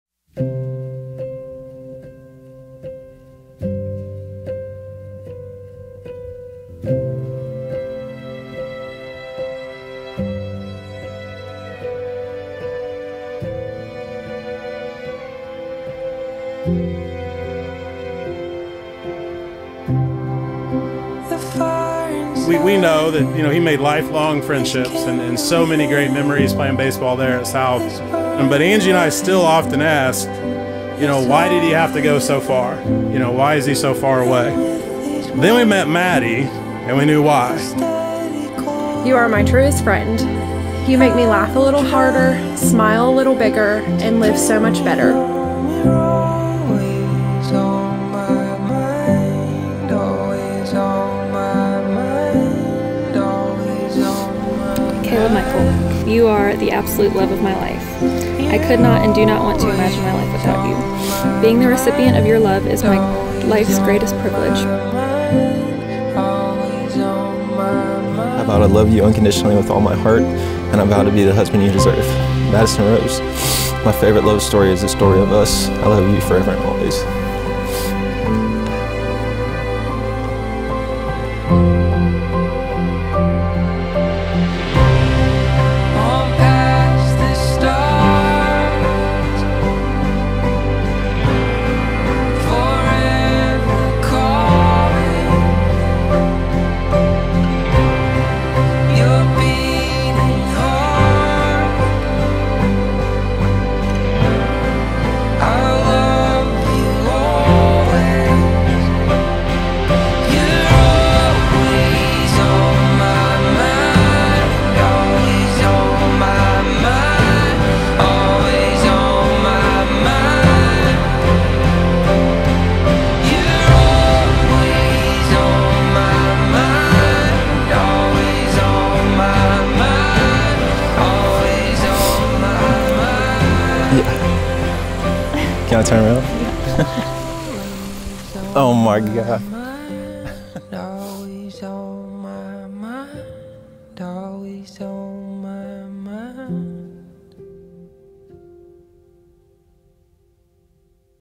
Eden Gardens State Park Wedding Video Teaser - Madi and Kaleb
Madi and Kaleb's big day at Eden Gardens was nothing short of magical ✨ The love these two have for each other and their warm and fun personalities were just infectious! And can we just pause for a second and talk about this dress?! 🤩

Can't wait to share their full film soon. Until then, here's a little sneak peek for you!

P A R T N E R S
Venue: Eden Gardens
Planning: Serene Occasions Wedding and Event Planning
Photography: Ashley Jen Photography
Hair: Hair by Caroline McDonald
Makeup: Alexisruby Artistry Collective
Dress: Bliss Bridal
DJ: Shane Keller Productions
Catering: Charcuterie by Shelli + Carrabbas
Cake: Cava and Cakes 30A

MB01WJIUNNN6QS1